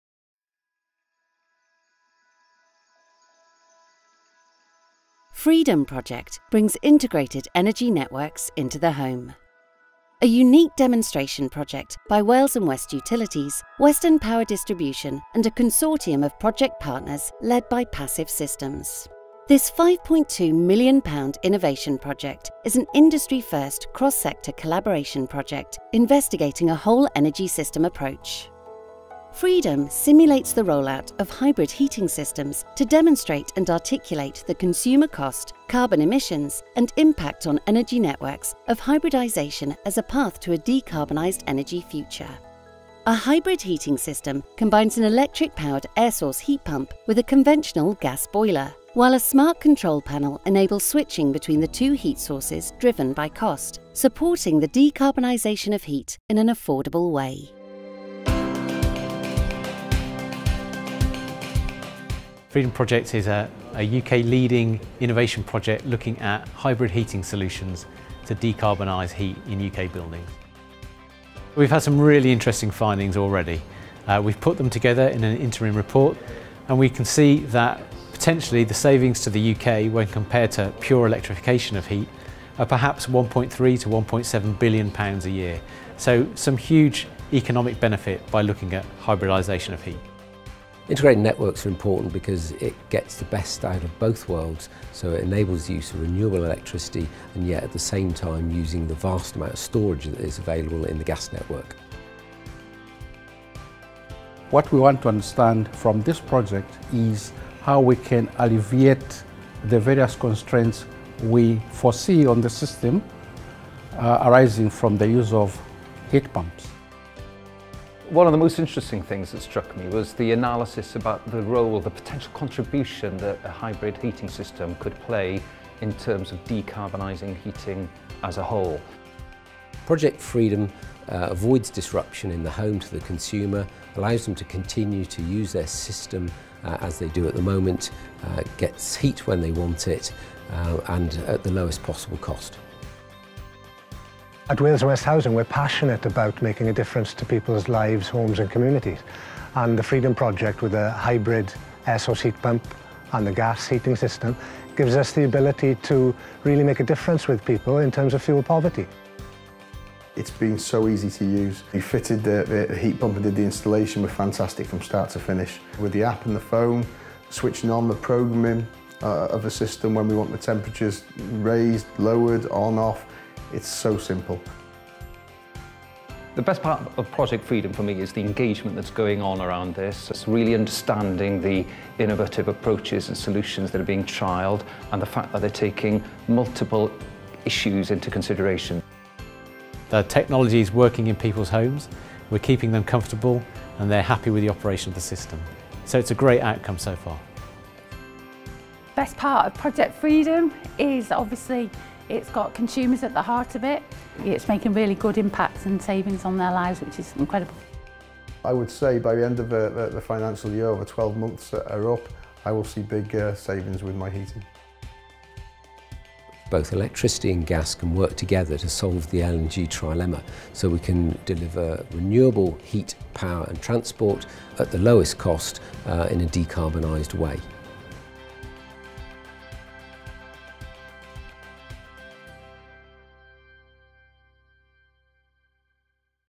Freedom - the future of home heating
We’re really excited about Freedom – a future of energy project we’re working on with Western Power Distribution and PassivSystems. It’s looking at the future of heating, so if you want to find out how you might be staying warm at home in the future, while keeping your bills and carbon emissions as low as possible, take a look at this short film!